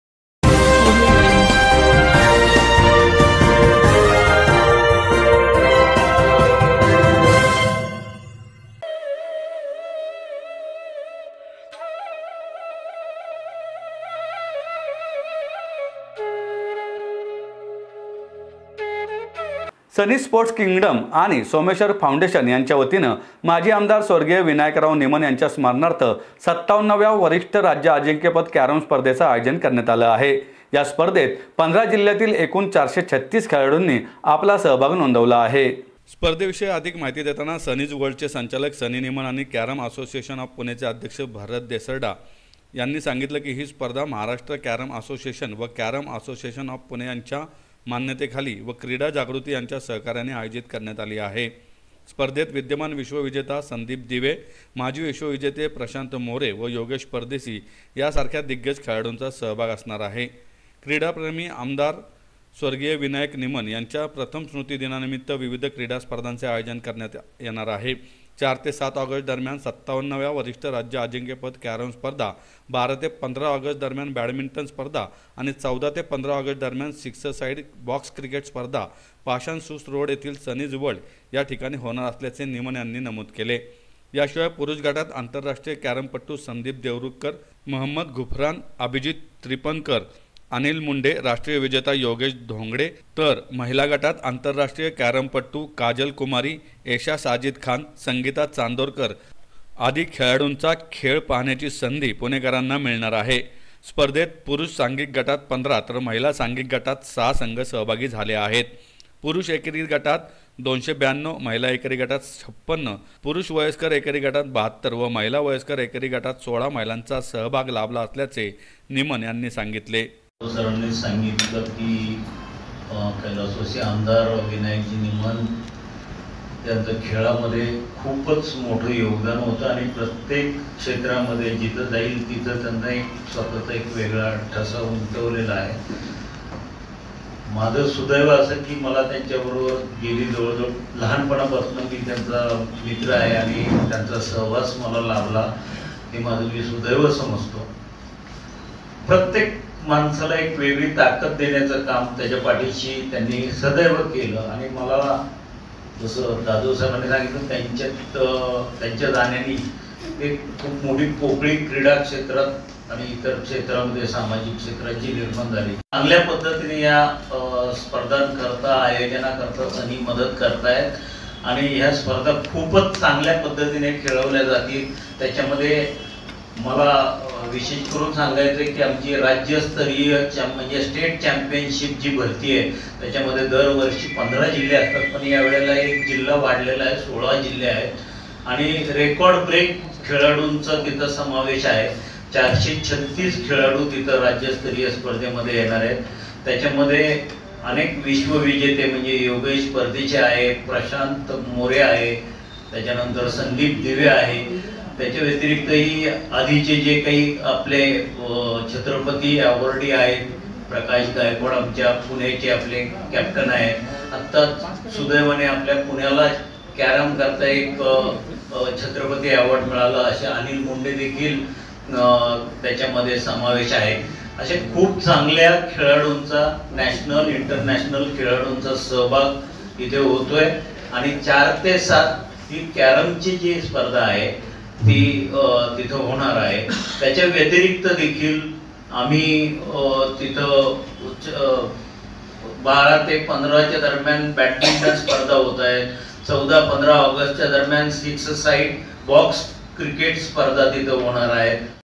कै विनायक निम्हण राज्य अजिंक्यपद कॅरम स्पर्धेत राज्यभरातून ४३६ खेळाडू सहभागी होणार
सनी स्पोर्ट्स किंग्डम आणि सोमेश्वर फाऊंडेशन यांच्या वतीने ayojan